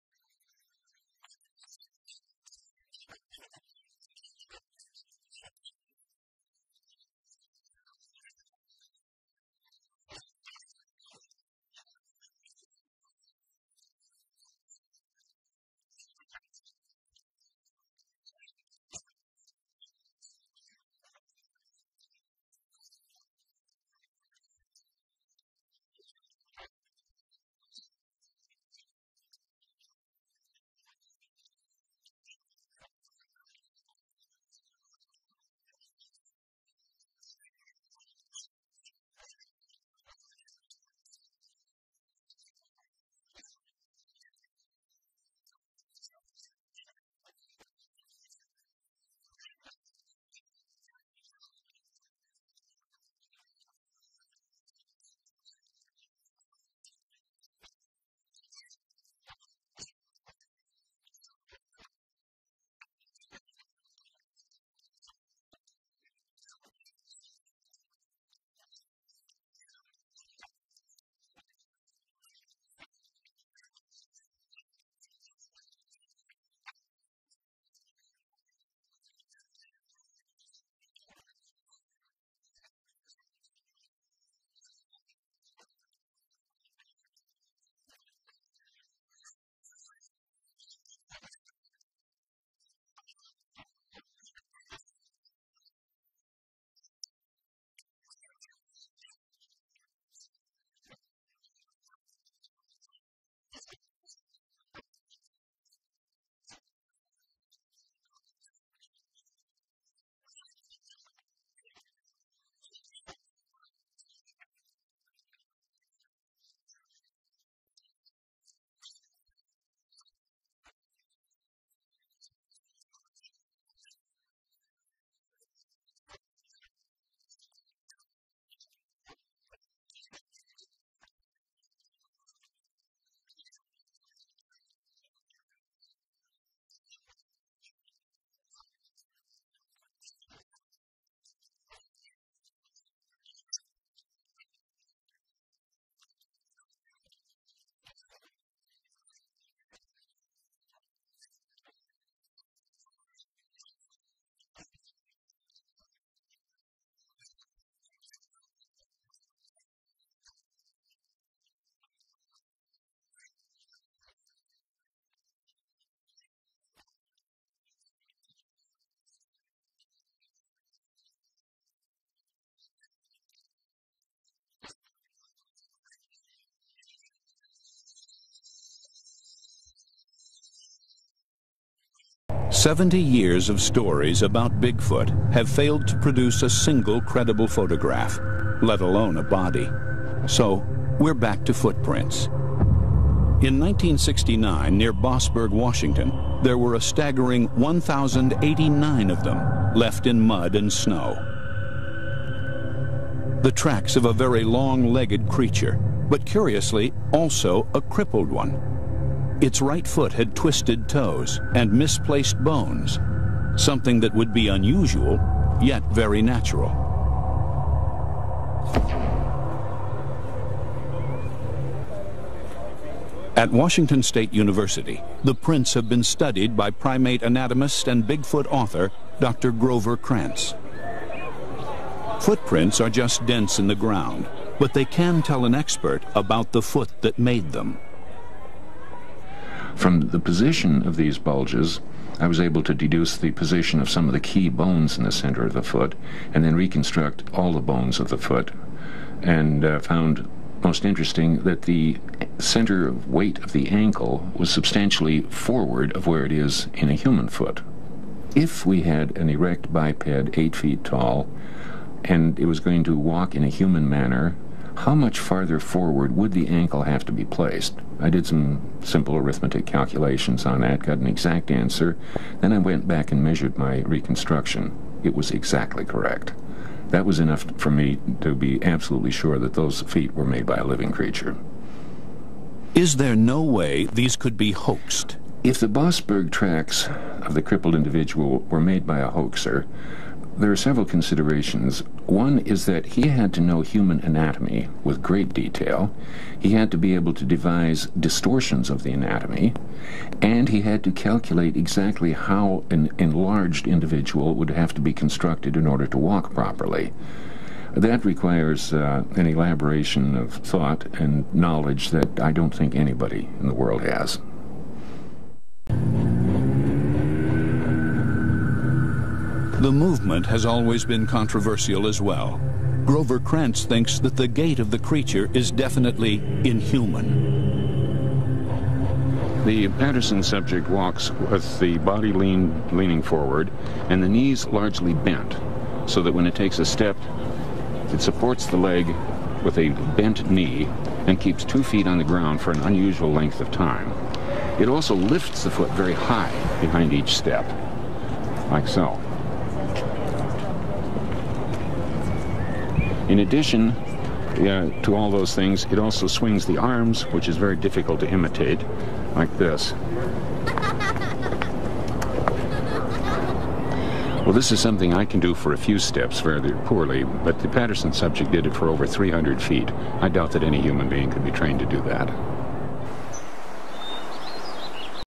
Anthropologist Grover Krantz on Bigfoot footprints
Clips of the late professor discussing Bigfoot evidence.